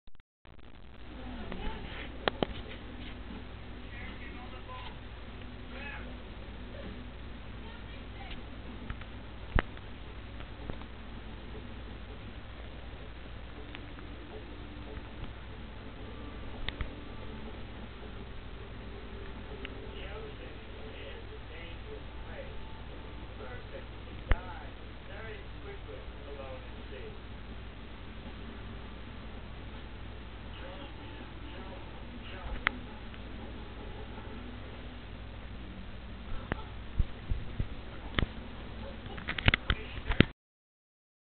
Puppy can't keep her eyes open
Katie the American Pit Bull puppy falling asleep and she can't keep her eyes open. Shes 7 wks in this video.  Shes a sweet baby!